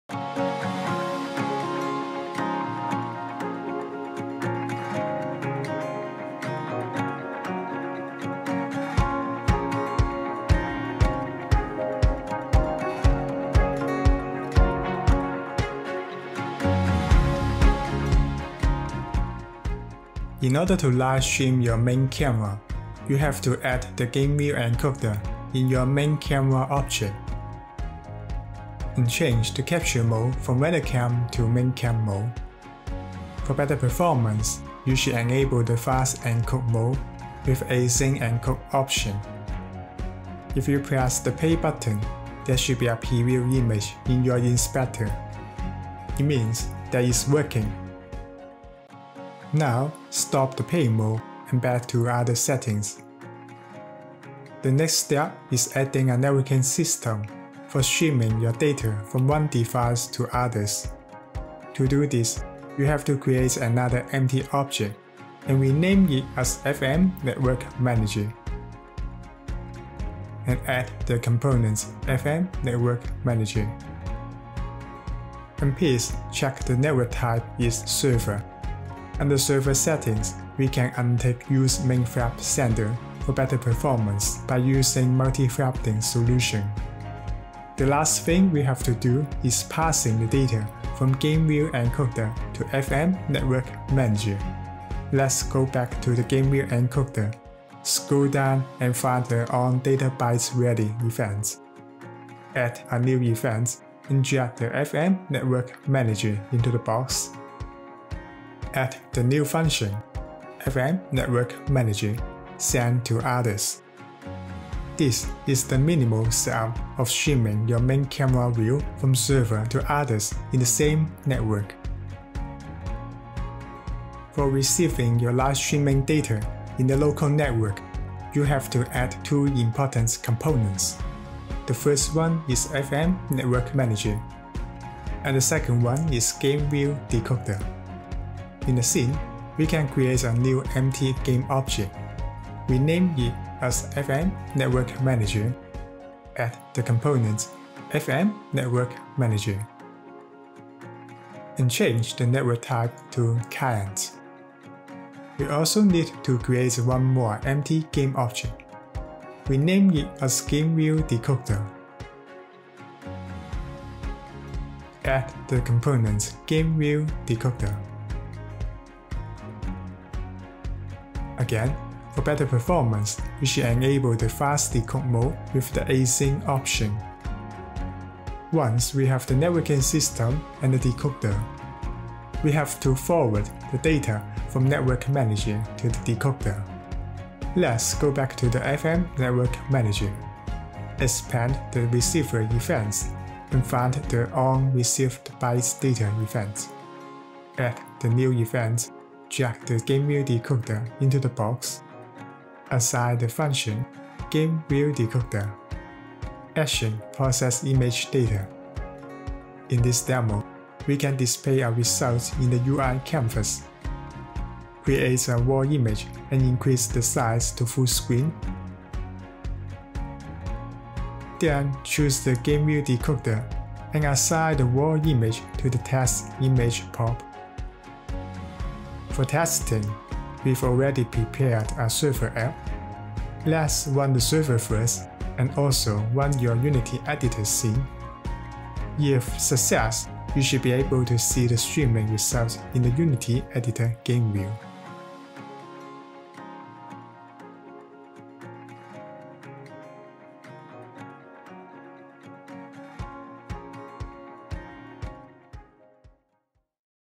[Tutorial: FMETP STREAM 3.0] (4mins) FM Network Stream (Basic)
FMETP STREAM is a plugin for Unity3D, which simplifies the workflow of live streaming without coding.

supported platform:
iOS, Android, Mac, PC, WebGL, HTML, UWP